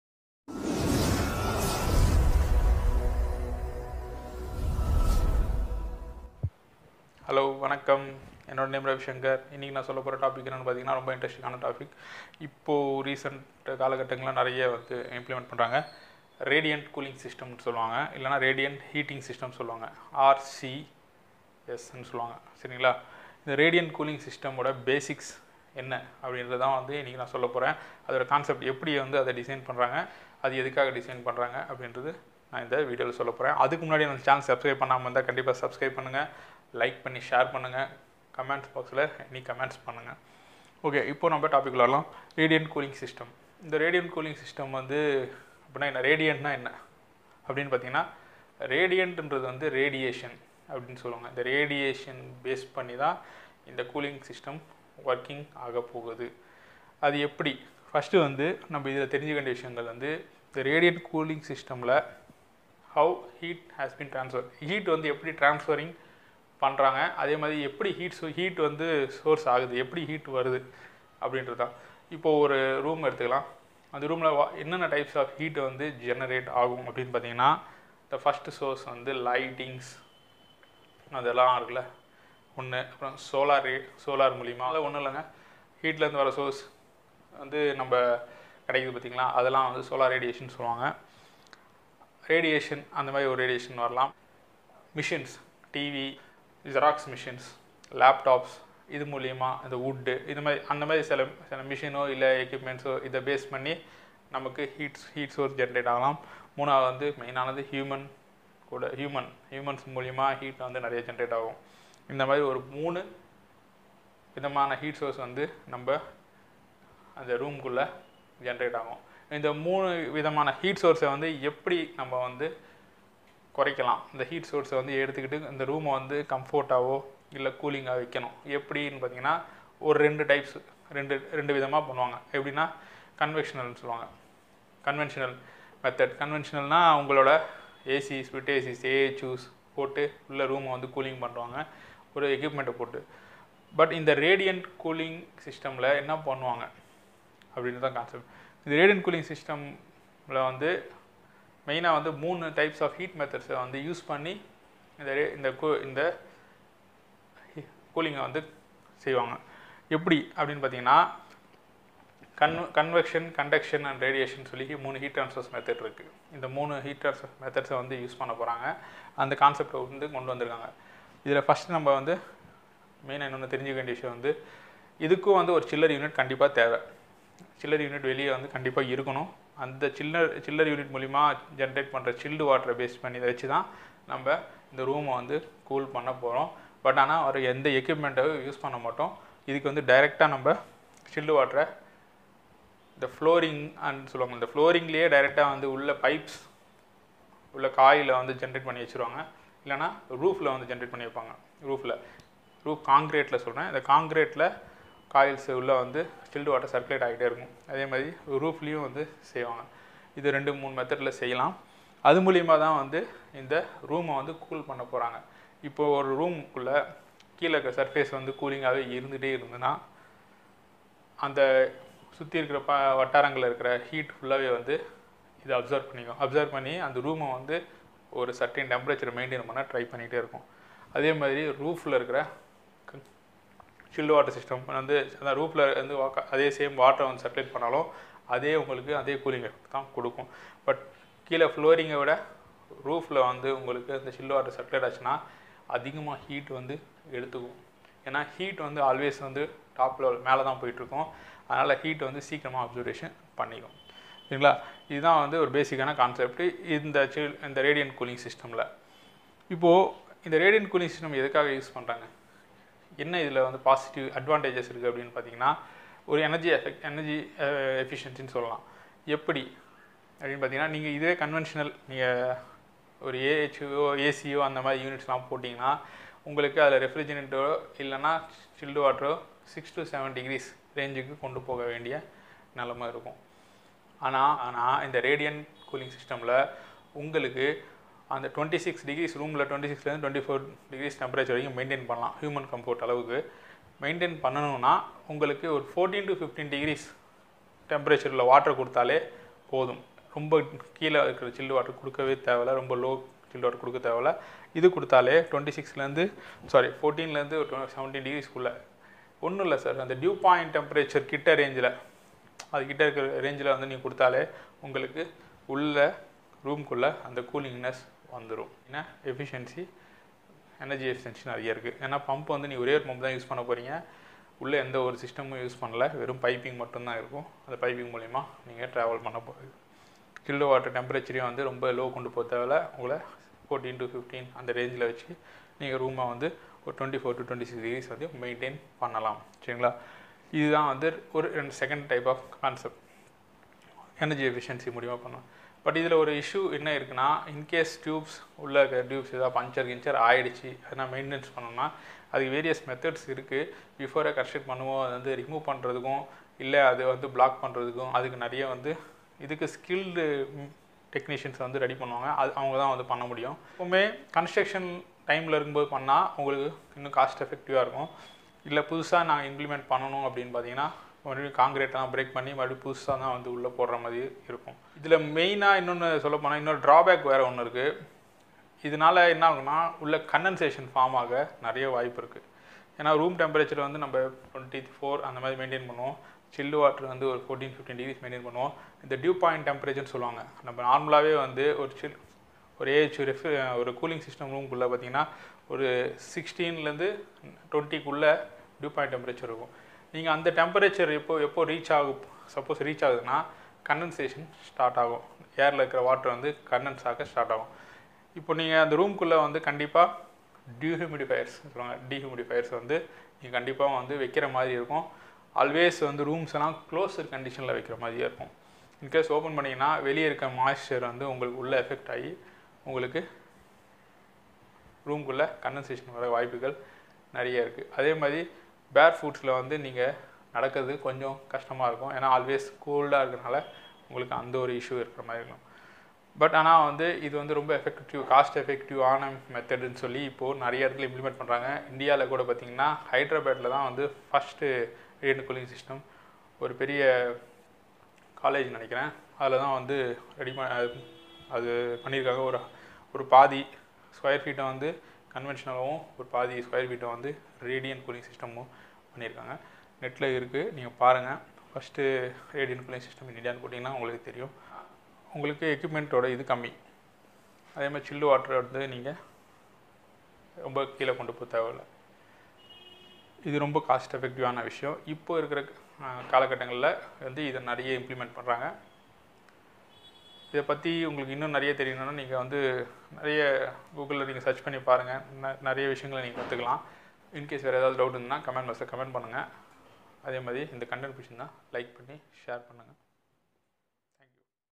Radiant Cooling System | HVAC | How radiant cooling system works | Tamil | Lohisya media
This video gives you the information about how radiant cooling system works explanation by Ravishankar in Tamil